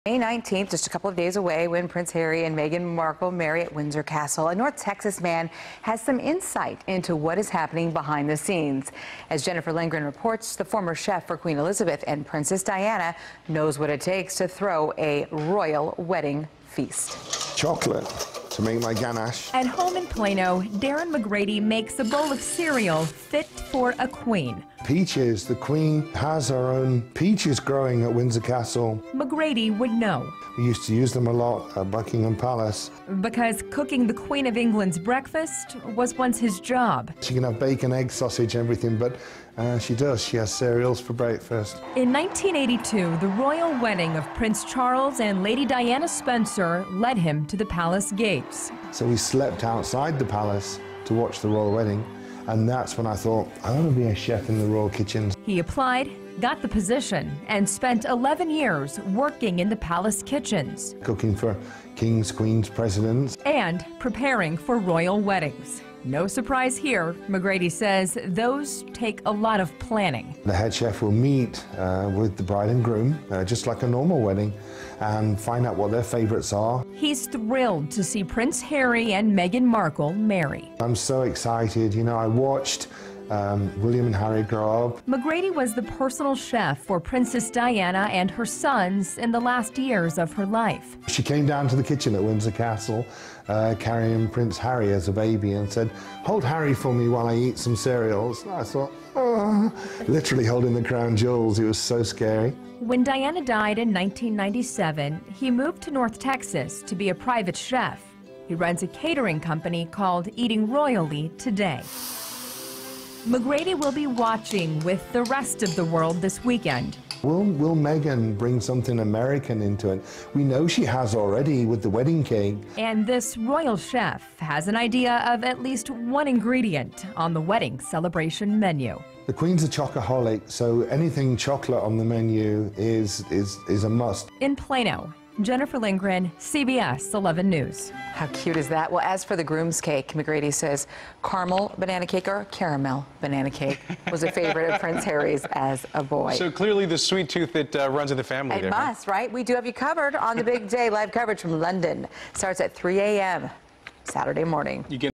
Royal Chef Looking Forward To Royal Wedding
Darren McGrady was at the Windsor Castle for 11 years, working in the Queen's kitchen, as her personal chef.